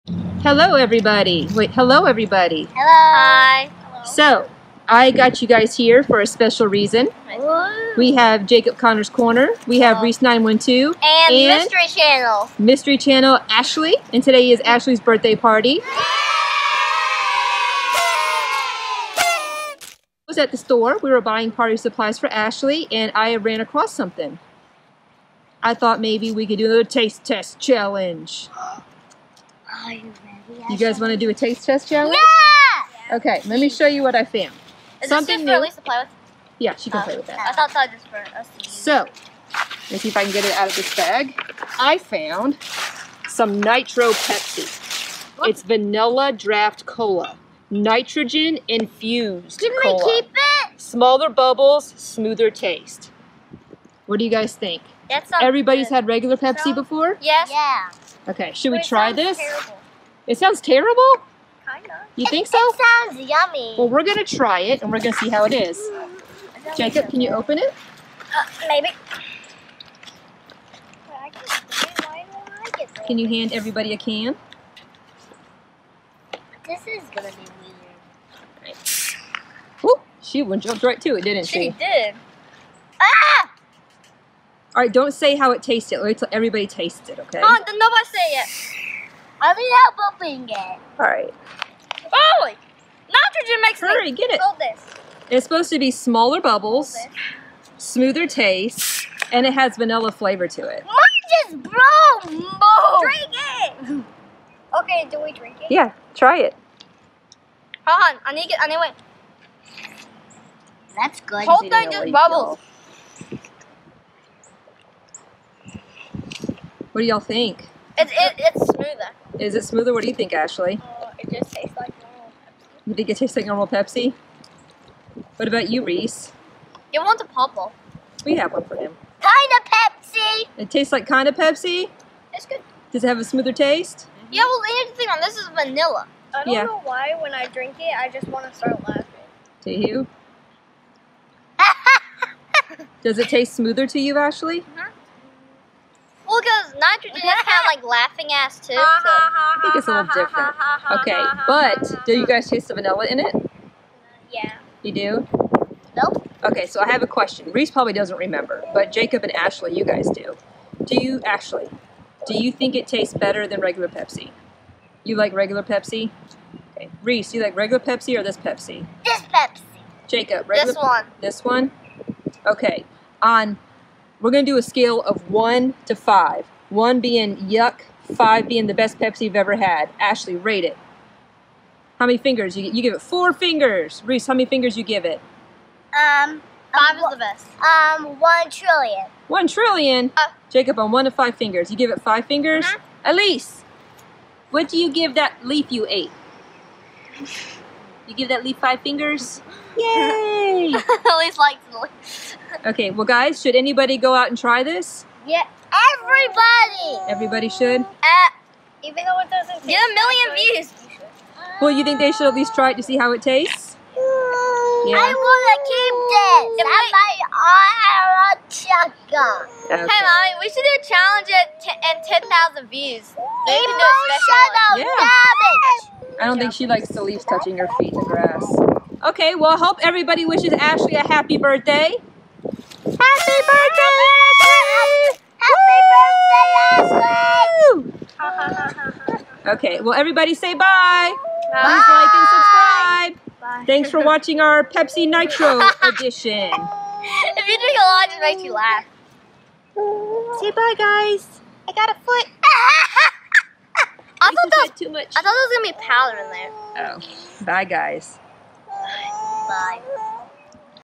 Pepsi Nitro Taste Test
We found a new Pepsi while out shopping for party supplies for Ashleigh's party. We got one for everyone and decided to do a Pepsi Nitro Taste Test! Let's go see if it is any good! And a very special HAPPY BIRTHDAY to Ashleigh!!!